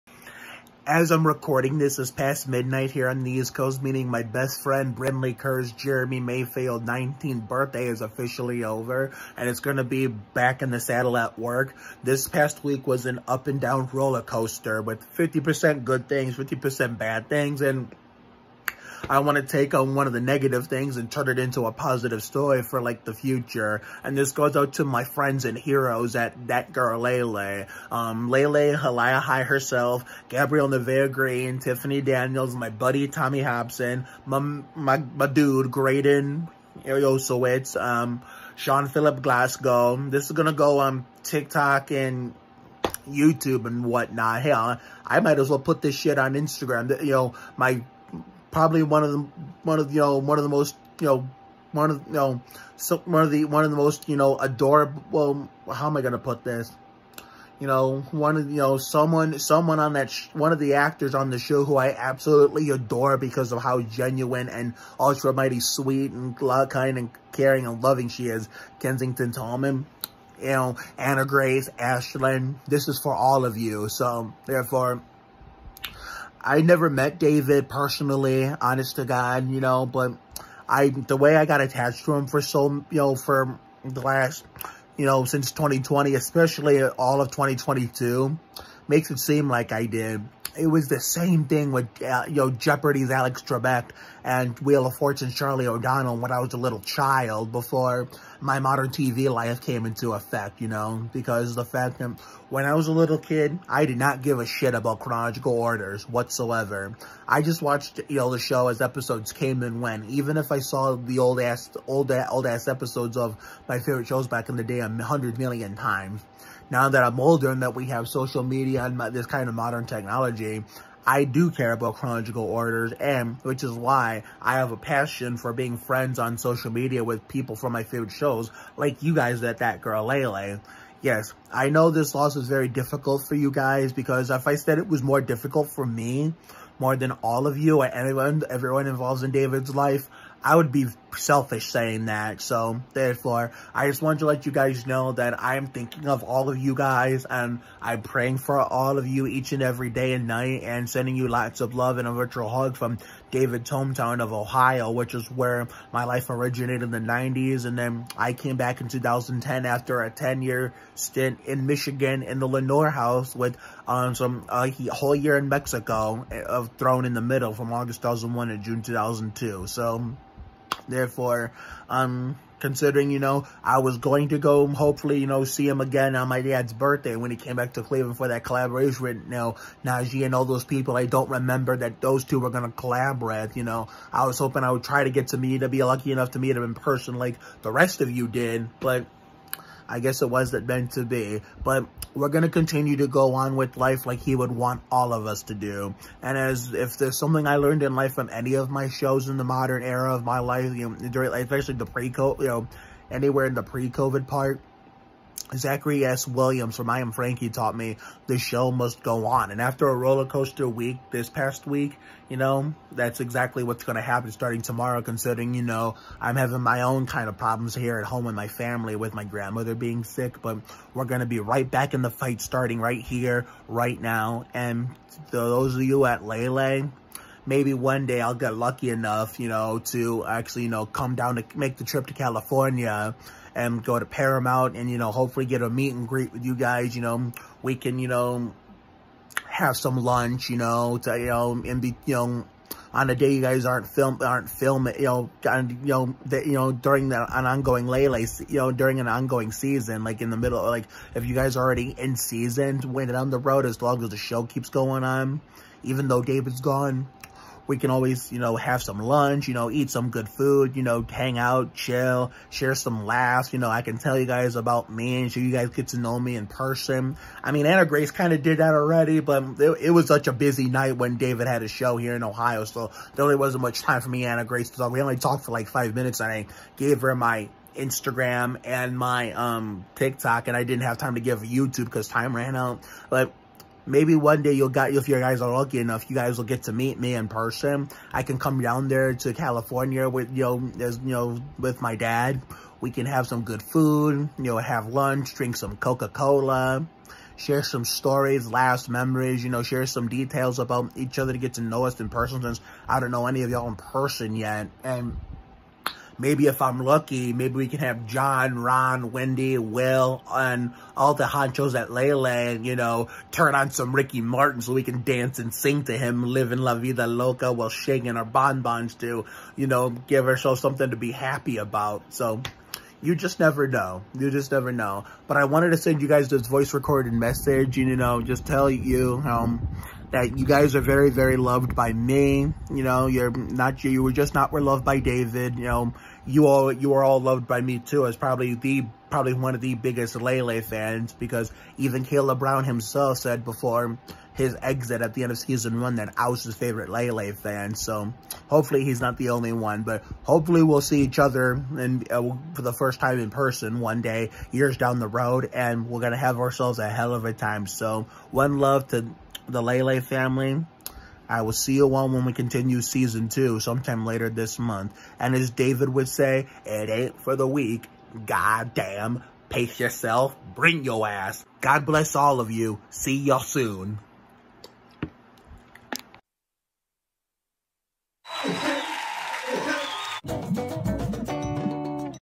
A Message to the Cast & Crew of That Girl Lay Lay | In Loving Memory of
A message to @annagracearnold74 (his older daughter), @gabriellenevaeh, @meetcalebbrown and the rest of the cast and crew of one of my favorite shows this man had the honor of bringing to life to lift their spirits up after what was a rough week for them and an up and down rollercoaster week for me!

As @ZacharySWilliams taught me on another @Nickelodeon Show back in 2018, the show must go on and that’s exactly what’s gonna happen this week and beyond as the season will also resume this month!

Hopefully one day I’ll be lucky enough to drive all the way to California, come to Paramount Studios and get to meet all of you in person all courtesy of my dad @eddieo.1958 and we can grab a meal, drink some Coca Cola, chat it up, share some laughs and best of all, dance to some Ricky Martin!

Y’all are in my prayers and I love y’all very much! Thinking of all of you each and every day as like the rest of you, there’ll always be a Fat Ballerina sized place in my heart as long as I live! For @DavidAArnold! 🙏🕊👑🐐❤️